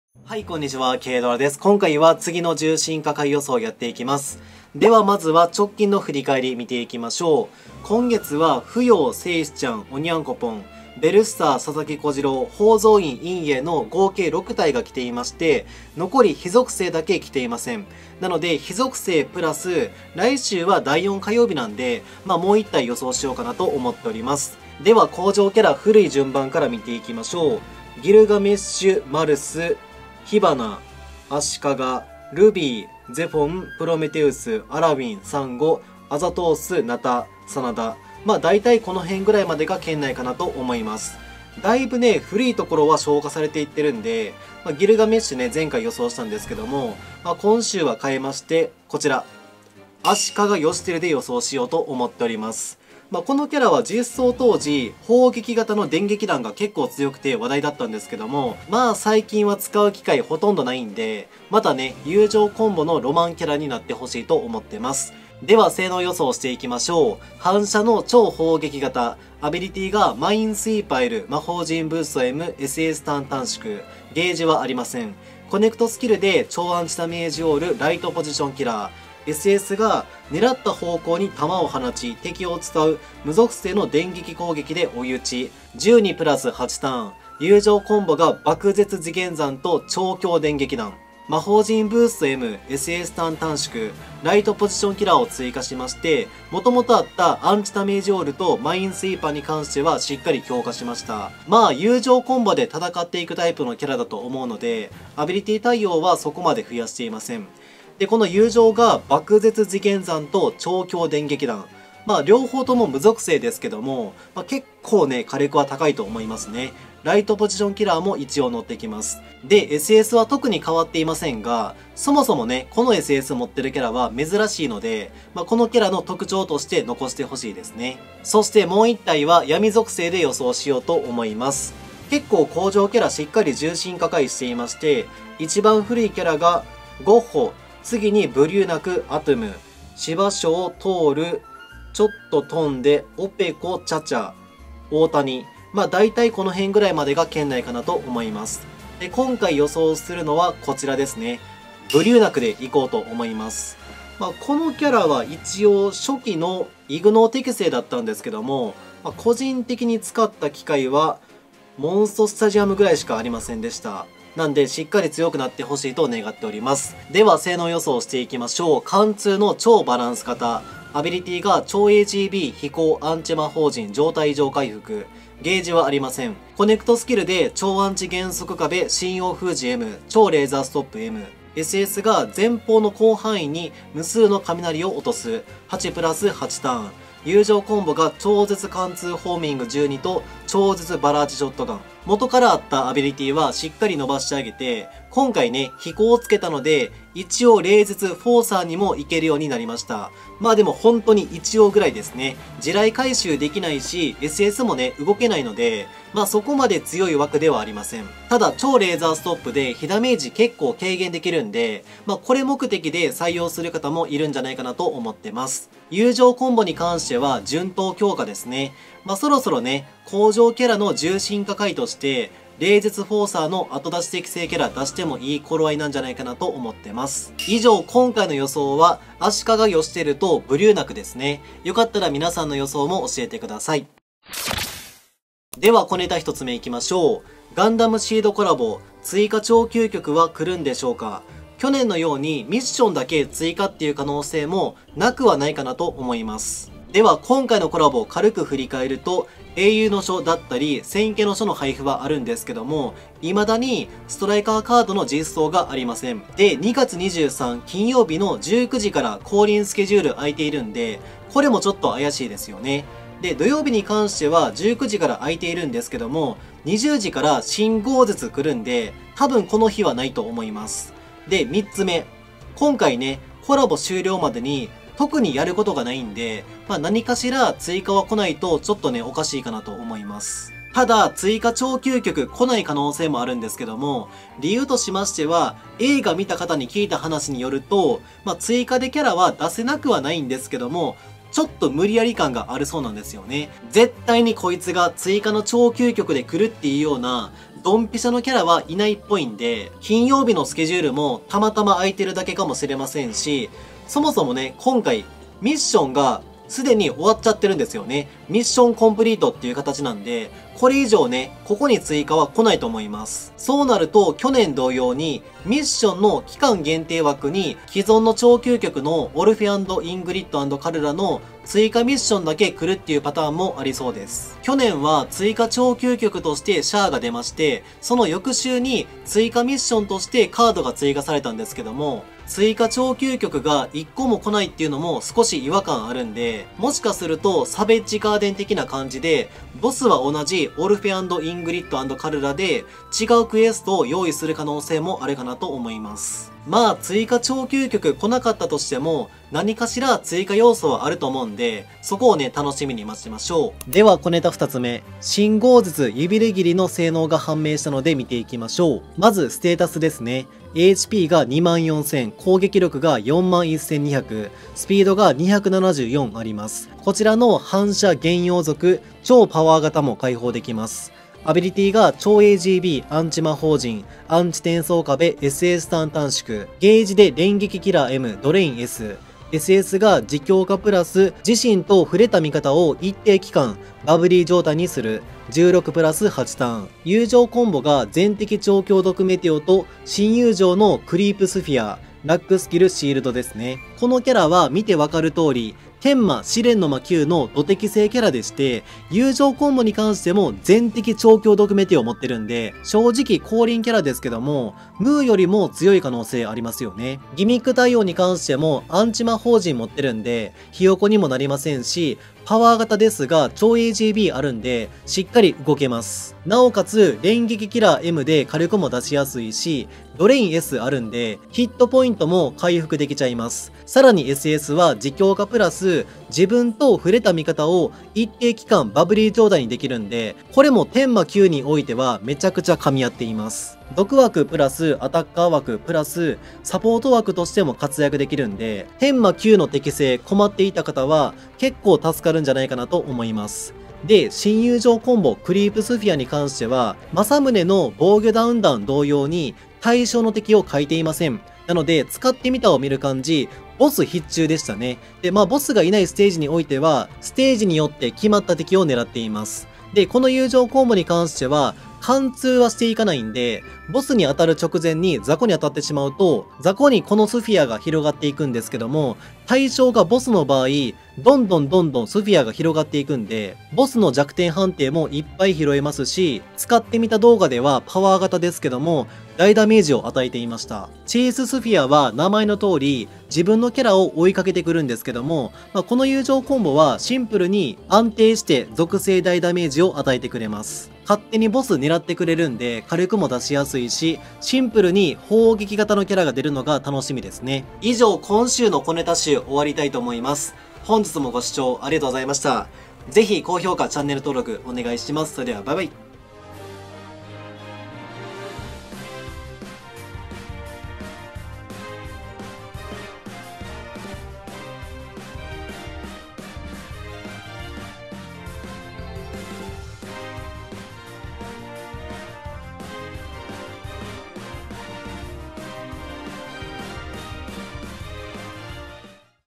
【今週の予想&小ネタ集】※ガンダムSEEDコラボ、追加超究極どうなる？ある説・ない説どちらも提唱。新轟絶ユビレギリの性能判明！天魔9ド適正、新友情「クリープスフィア」もボス必中でヤバそう【けーどら】
0:00 改予想①
2:08 改予想②
3:55 小ネタ①
5:53 小ネタ②

◆撮影/編集/企画/サムネ：けーどら